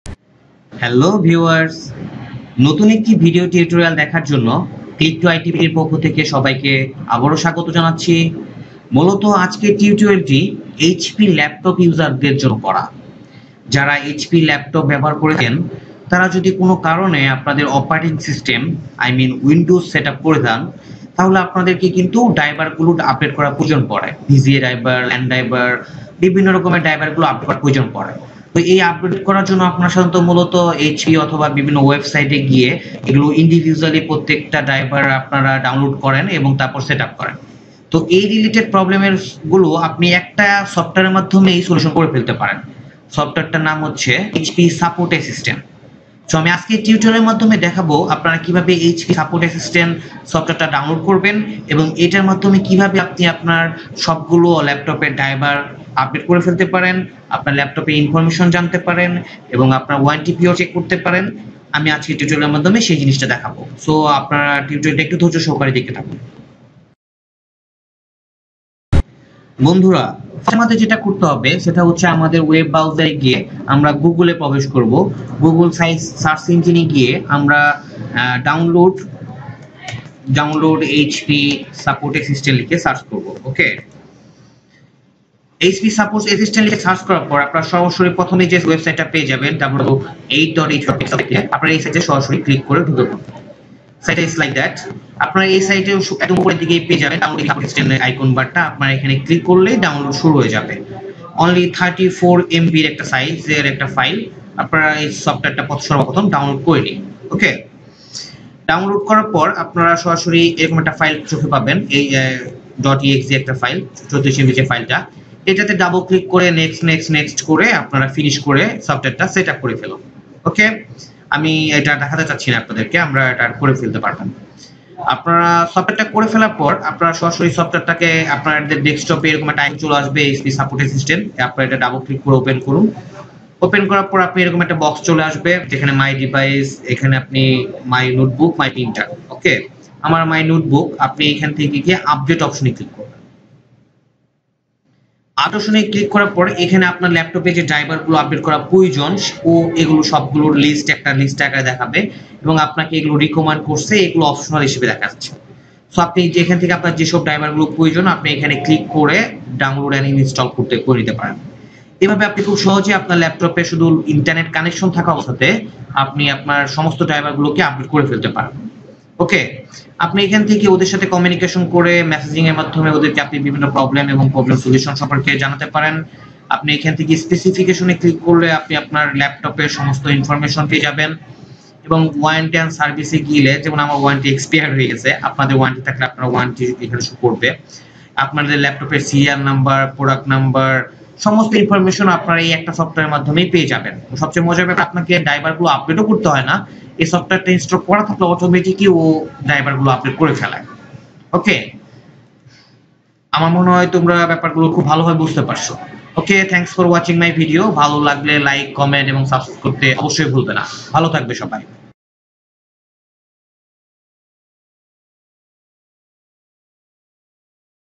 Install HP Laptop Drivers with HP Support assistant
Install HP Laptop Drivers with HP Support assistant.  This video helps you to know " how to update drivers of Hp laptop with HP Support assistant"